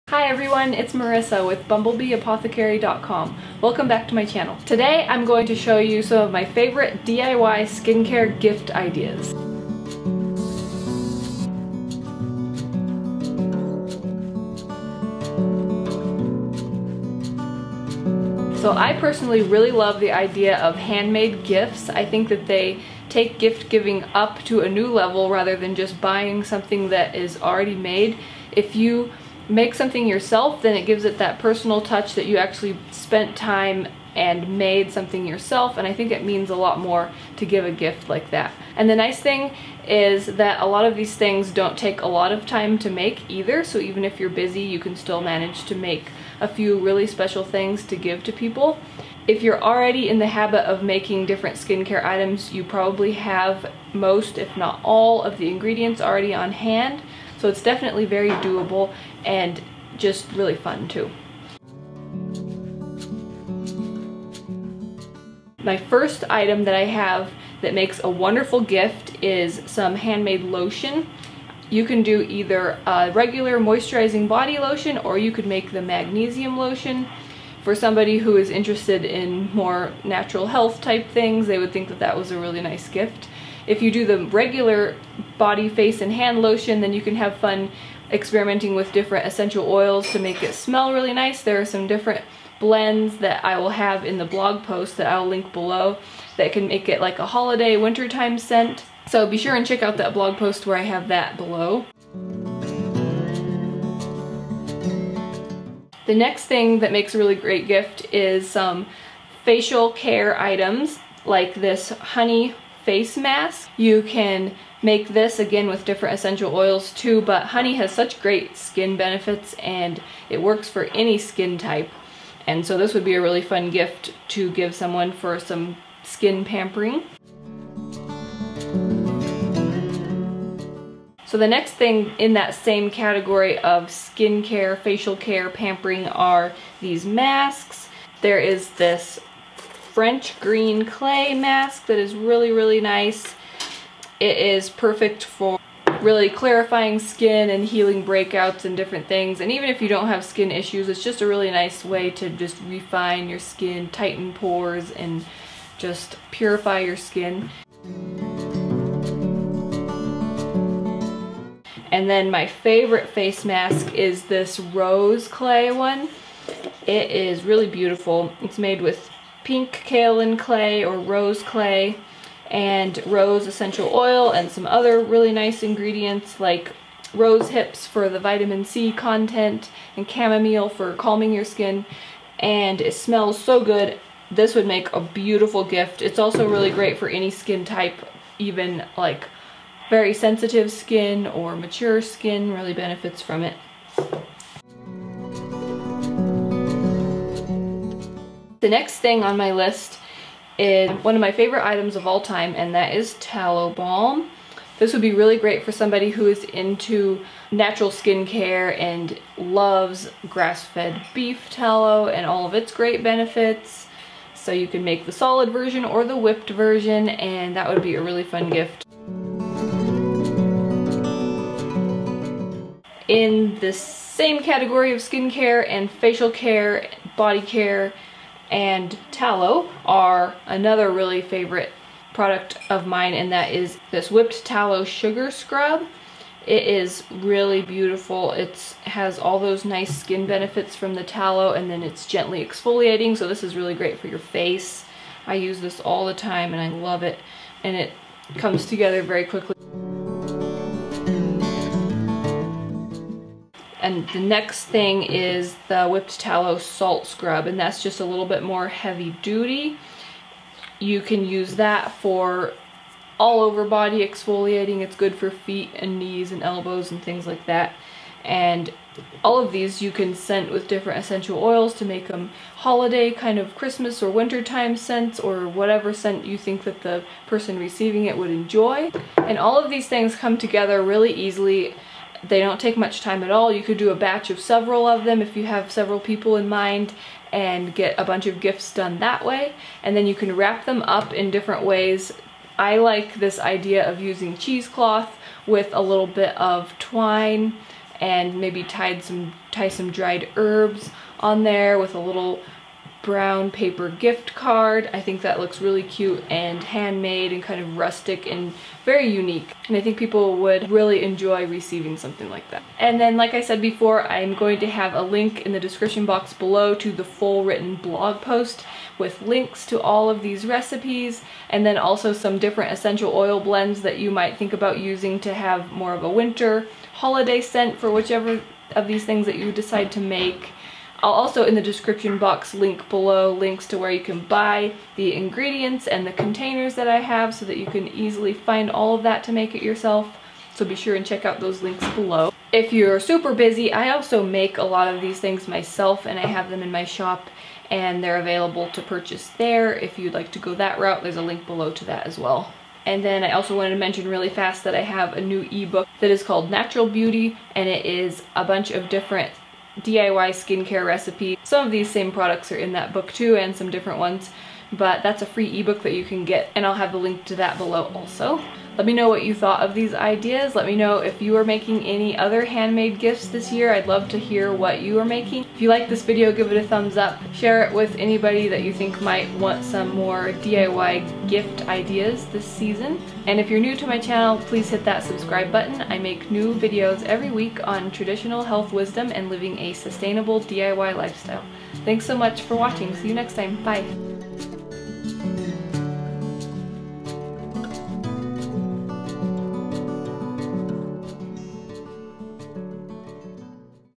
DIY Skincare Gift Ideas | 8 EASY RECIPES | Bumblebee Apothecary
DIY SKINCARE GIFT IDEAS | 8 EASY RECIPES // Handmade gifts are the best. You can personalize and customize them, and they're something you can't buy in any store. Here are my top DIY skincare gift ideas for that perfect, unique gift. Anybody who is into organic skincare will love these.

🐝 SHOP THIS VIDEO:
Grass fed tallow
Olive oil
Shea butter
Magnesium brine
Raw honey
Rose hips
Pink clay
French green clay
Glass jars

🐝 My current video equipment:
Canon EOS 80D

-Marisa 🐝